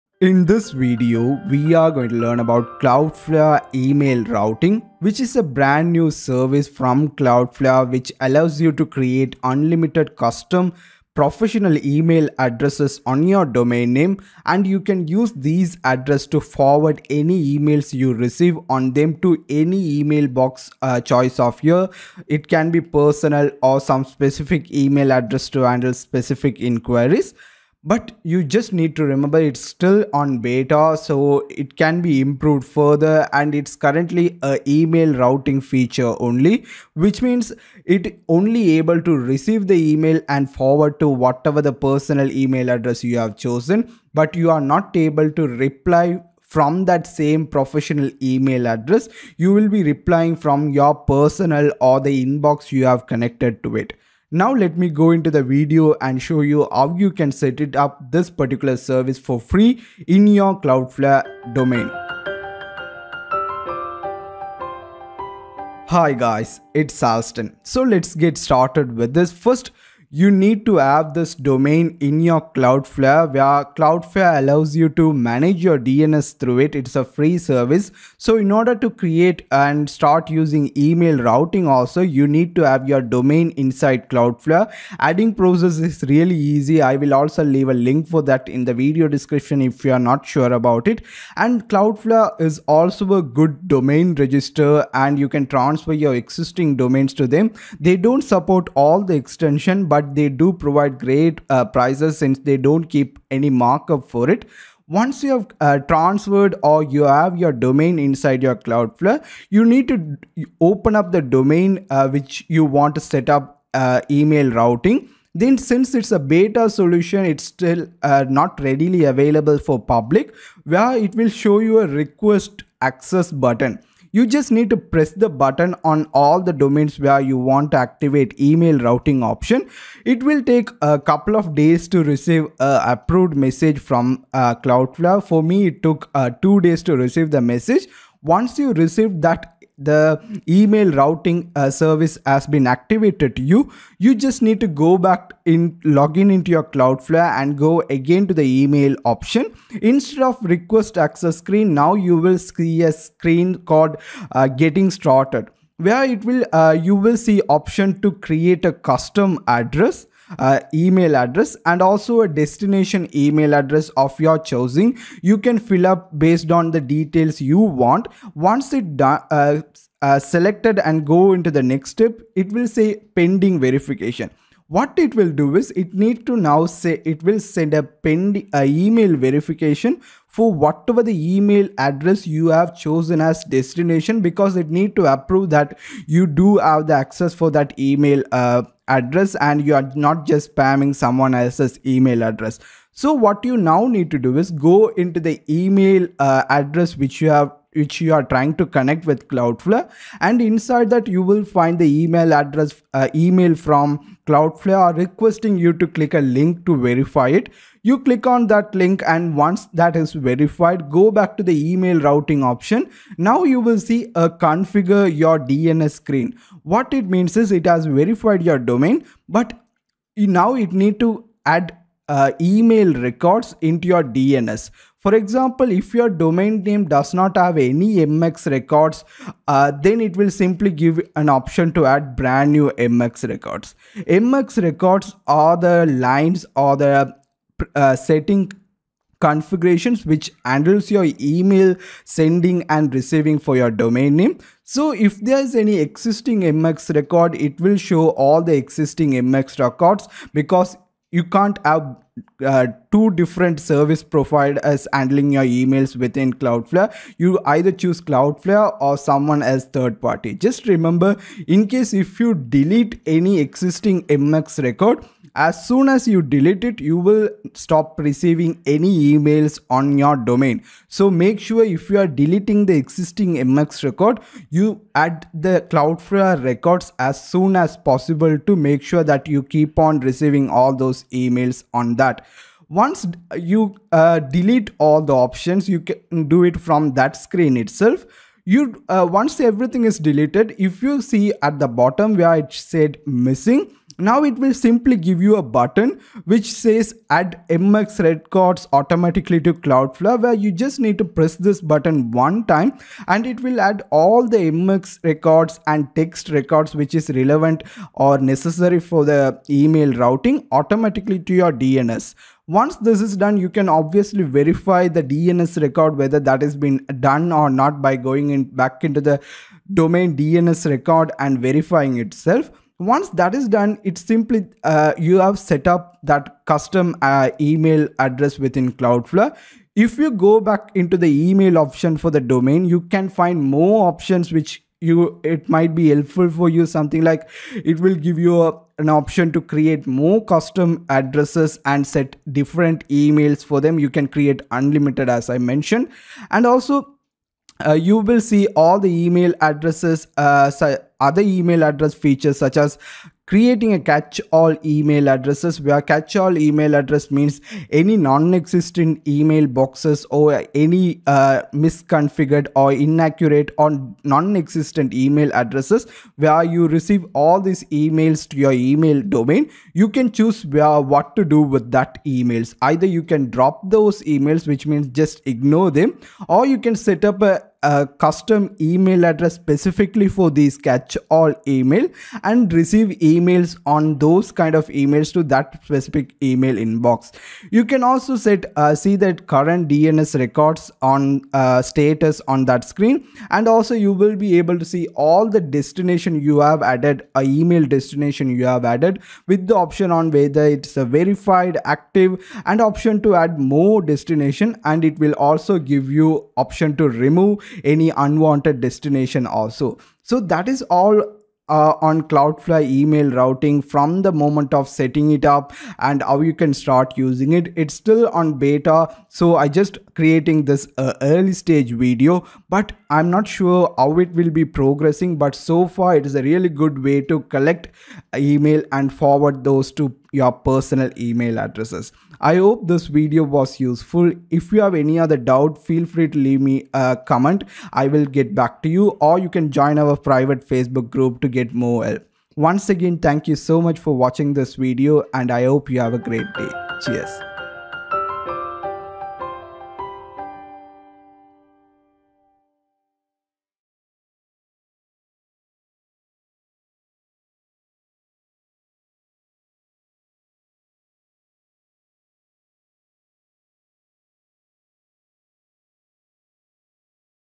Free Cloudflare Email Routing: Branded Inbox That Helps Your E‑E‑A‑T
In this video we will learn how to use Cloudflare email routing and set up professional custom email address which will forward email messages to your personal or any email address like Gmail.

00:00 Introduction to Cloudflare Email Routing
01:09 Cloudflare Introduction
01:49 Requesting Email Routing
02:10 Cloudflare Verification
02:24 Creating Custom Email Forwarders
02:50 Verify Destination Email Address
03:30 Setup Email MX Records
05:12 Verifying MX Records
05:22 Email Routing Options
05:35 Creating More Custom Addresses
05:46 Setup Catch-all Email Address with Drop or Forward Option
06:27 Managing Destination Addresses
06:50 Conclusion

It can also help establish credibility and trust.

It also helps you reduce spam, protect your privacy, look professional, avoid data scraping of main email addresses, and more.

But what if you don’t pay for one.

That’s where the Cloudflare email routing service is suited to you.

What is Cloudflare’s Email Routing service?
Cloudflare is a DNS and DDoS protection service that offers a range of services, including domain registration features & email routing.

No need for complicated web hosting control panels or email hosting setup processes.